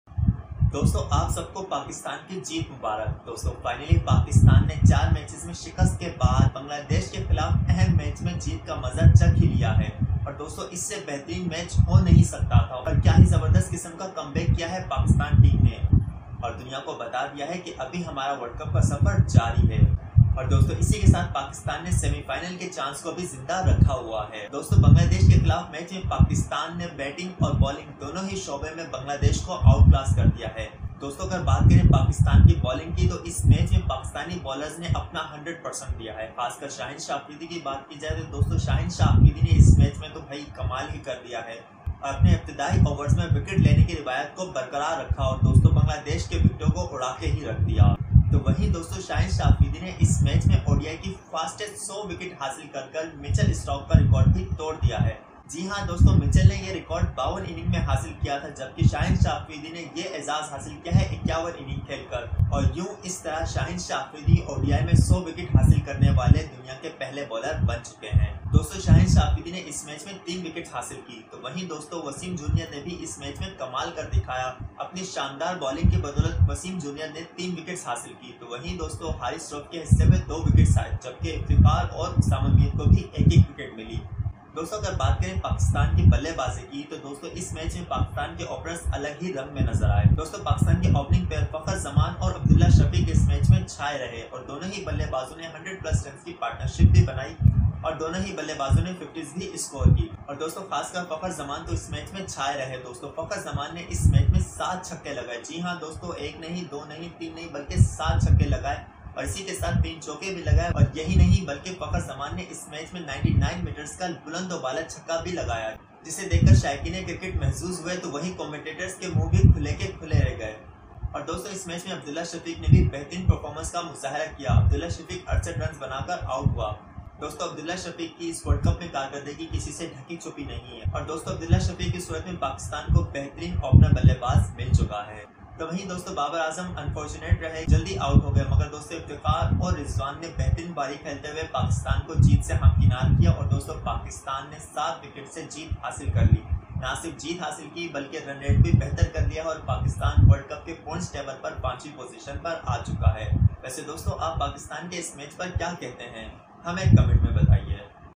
Fakhar Zaman Rocks | Pak vs Ban
Bangladesh Exits Tournament as Shaheen Shah Afridi and Fakhar Zaman Lead Pakistan to Resounding Victory. Pakistan, despite four consecutive losses, now stands at No. 5 on the points table, kept their faint hopes of a semi-final appearance alive.

Shaheen Shah Afridi's stellar performance, securing his record as the quickest quick bowler to reach 100 ODI wickets and the leading wicket-taker of this World Cup, paved the way for Bangladesh's dismissal at 204. Freshly returned to the squad, Fakhar Zaman showcased his best form in months with a classic 81 off 74 balls, catapulting Pakistan to victory. This win not only boosted their confidence but also significantly improved their net run-rate.